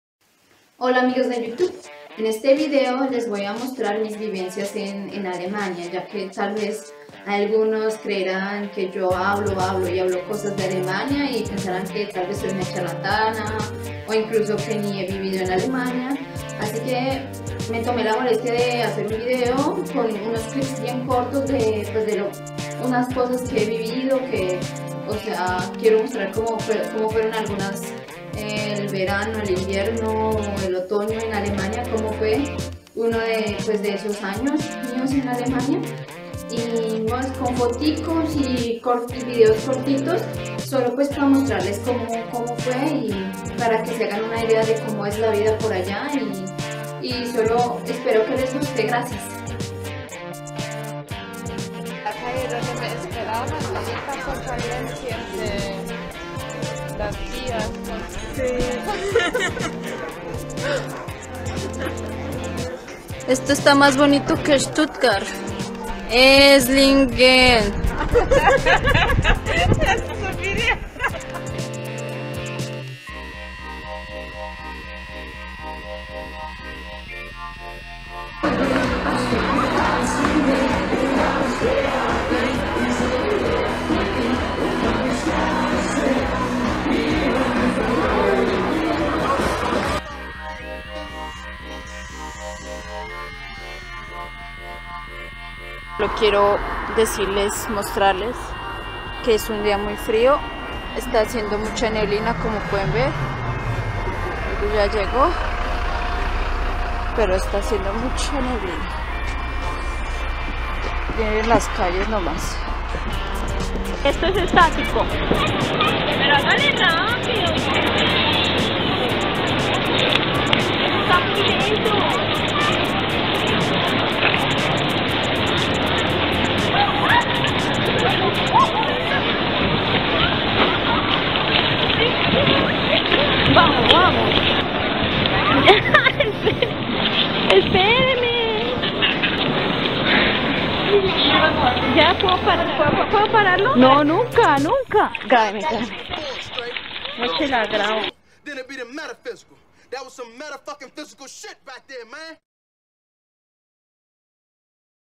mi vida en alemania
mezcla de videos cortos y fotos de mi vida en alemania